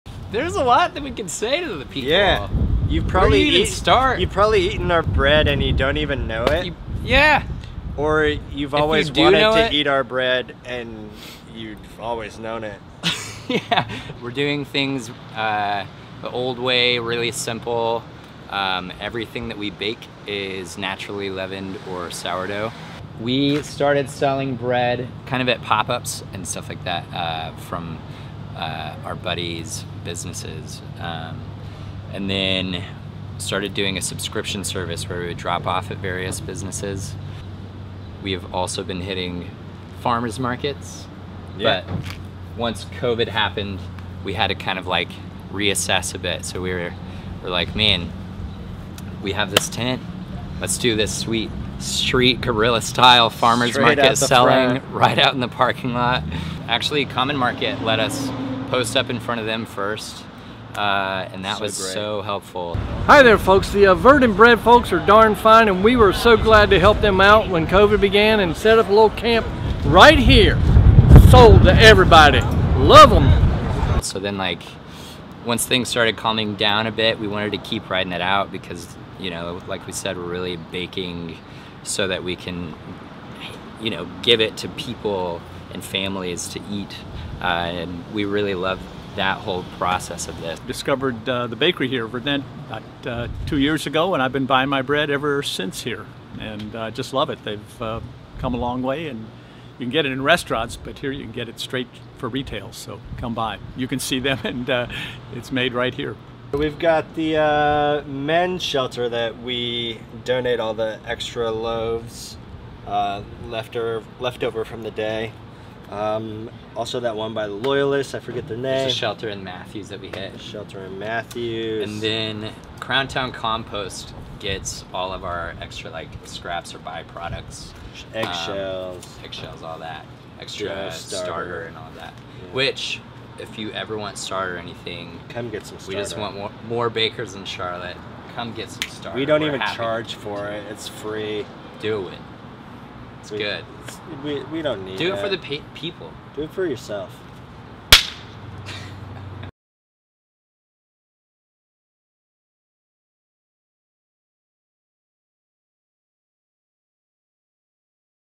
Find Freshly Baked Bread At Verdant Bread In Charlotte, Nc
The bakery tucked behind Common Market Oakwold in Charlotte, NC offers sourdough, baguettes, gluten-free bread and more to farmers markets, restaurants and by subscription.

Video by Alex Cason Photography/The Charlotte Observer